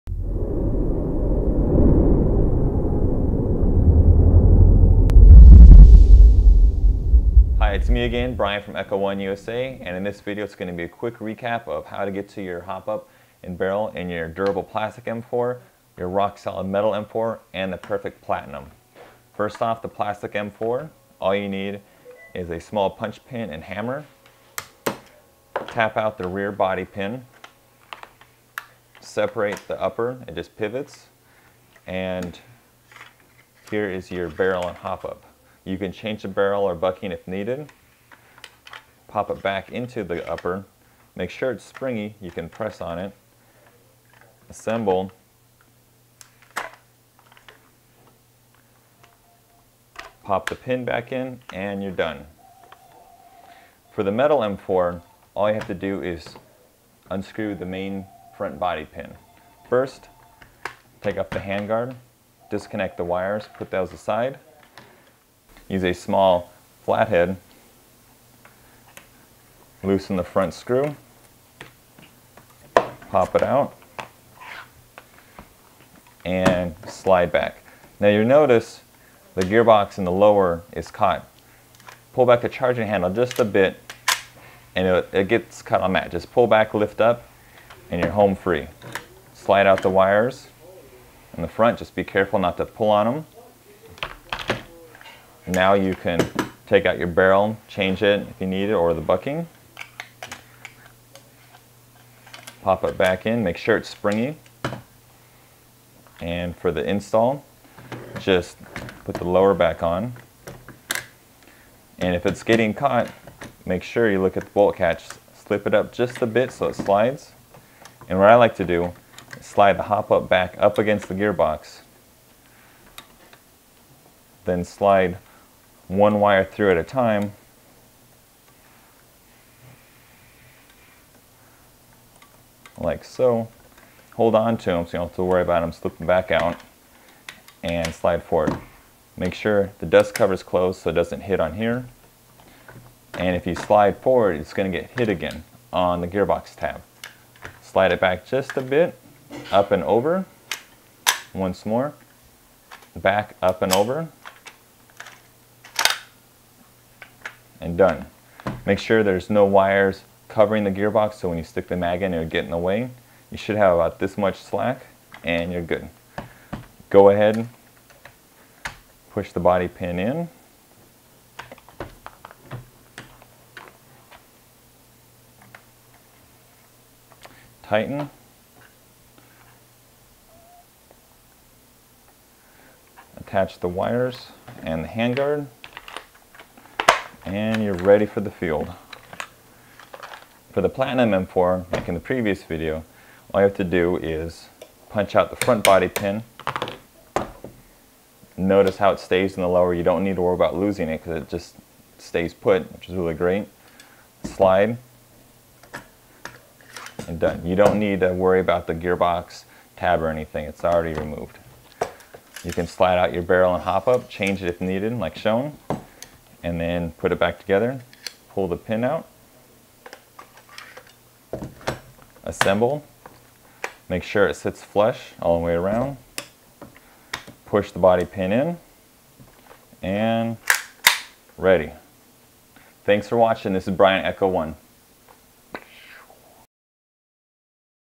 Echo1USA Tech Videos - Echo1 M4 Series Barrel Replacement Tutorial
Brian, the Echo1USA Tech, shows you how to replace / upgrade the barrel on the M4 series - Plastic Stag Arms, Metal Body Stag Arms, and Platinum Series M4.